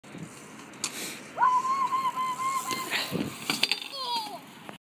Funny BMX crash
Filmed at Hinton skatepark! Rider is Jaiden Gibson